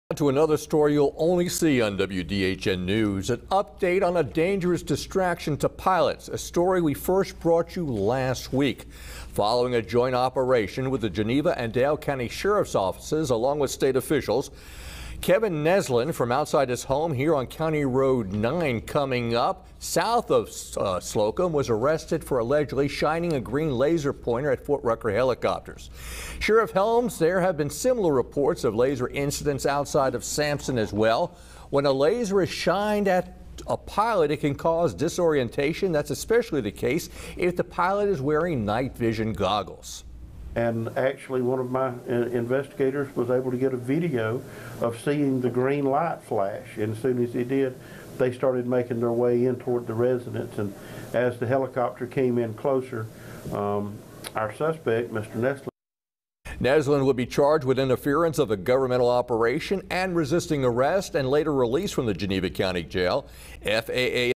Laser light pointed toward Ft. Rucker helicopters
Geneva County officials say they have received additional calls from a person or persons shining lasers at military helicopters flying over rural areas.